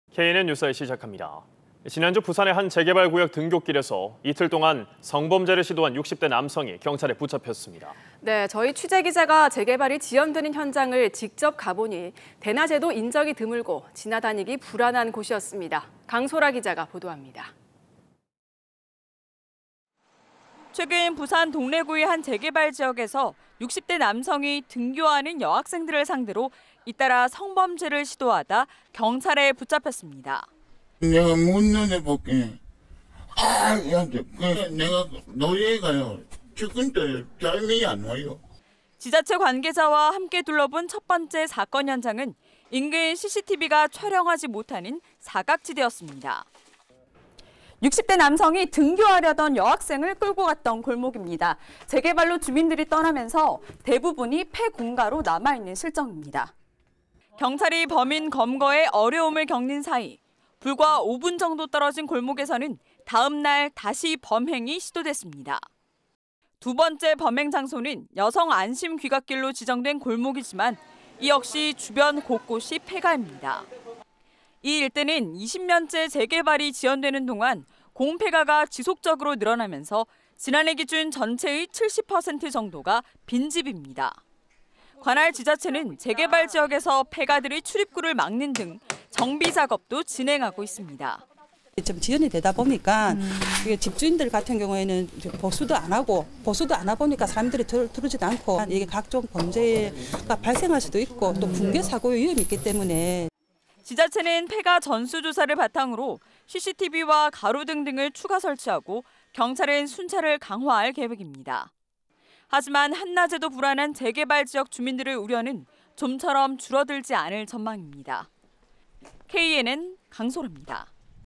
여성안심 귀갓길도 '안심 못해'
[앵커]
지난주 부산의 한 재개발구역 등굣길에서 이틀 동안 성범죄를 시도한 60대 남성이 경찰에 붙잡혔습니다.

취재기자가 재개발이 지연되는 현장을 직접 가보니, 대낮에도 인적이 드물고 지나다니기 불안한 곳이었습니다.

강소라 기자가 보도합니다.

[기자]
최근 부산 동래구의 한 재개발 지역에서는 60대 남성이 등교하는 여학생들을 상대로 잇따라 성범죄를 시도하다 경찰에 붙잡혔습니다.

{목격 주민"내가 문을 열어보니까 (학생이)악! 하더라구요. 내가 놀라서 지금도 잠이 안와요."}

지자체 관계자와 함께 둘러 본 첫 번째 사건 현장은 인근 CCTV들이 촬영하지 못하는 사각지대였습니다.

60대 남성이 등교하던 학생을 끌고가려던 골목길입니다.

재개발로 주민들이 떠나며 대부분이 폐가로 남아있는 실정입니다.

경찰이 범인 검거에 어려움을 겪는 사이, 불과 5분 정도 떨어진 골목에서는 다음 날 다시 범행이 시도됐습니다.

두번째 범행 장소는 여성안심 귀갓길로 지정된 골목이지만 이 역시 주변 곳곳이 폐가입니다.

이 일대는 20년째 재개발이 지연되는 동안 공폐가가 지속적으로 늘어나면서 지난해 기준 전체의 70% 정도가 빈 집입니다.

관할 지자체는 재개발지역에서 폐가들의 출입구를 막는 등 정비작업도 진행하고 있습니다.

{주민센터 관계자"(재개발)지연이 되다보니까 집주인들 같은 경우에는 보수도 안하고 보수도 안하다보니까 사람들이 들어오지도 않고...각종 범죄가 발생할수도 있고 또 붕괴사고의 위험이 있기때문에"}

지자체는 폐가 전수조사를 바탕으로 CCTV와 가로등 등을 추가 설치하고, 경찰은 순찰을 강화할 계획입니다.

하지만 한 낮에도 불안한 재개발 지역 주민들의 우려는 좀처럼 줄어들지 않을 전망입니다.

knn강소라입니다.